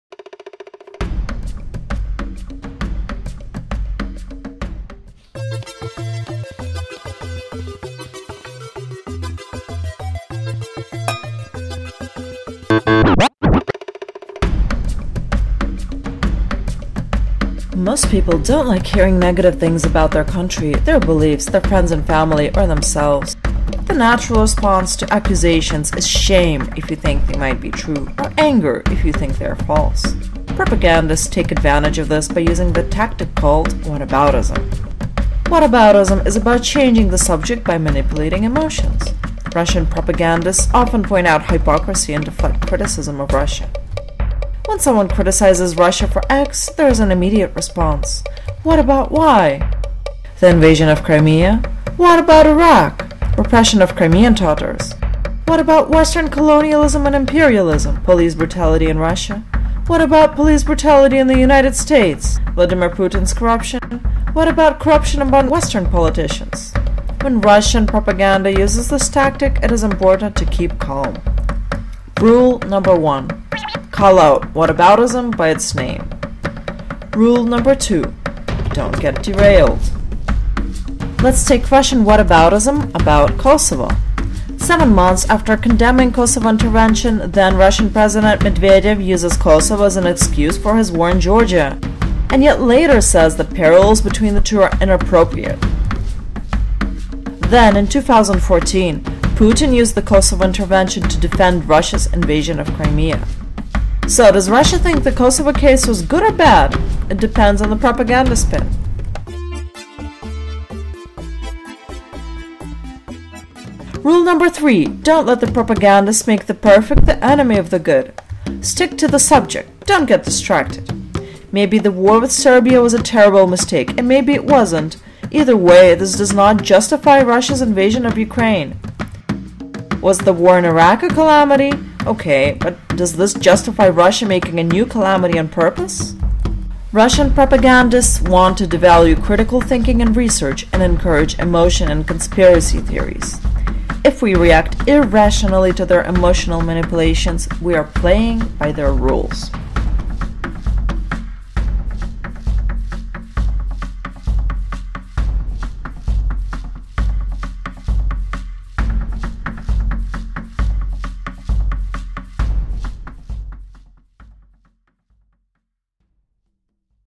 A guide to Russian propaganda. Part 2: Whataboutism
Video designer: Ganna Naronina; video script and idea: Alex Leonor, Alya Shandra

Whataboutism is alive and strong today in Russian propaganda. The basic mechanism is an attempt to use either the emotions of shame or anger to derail an argument. By accusing their opponents or even just their interlocutors of hypocrisy, propagandists hope to trigger the emotion of shame to blunt accusations or divert the conversation into a discussion about hypocrisy. If the counter-accusation the propagandist uses is absolutely ridiculous, as is often the case,  then the propagandist is probably not trying to use shame, but rather use one’s anger to divert the conversation away from an important matter at hand and into a constant back-and-forth about some irrelevant piece of history.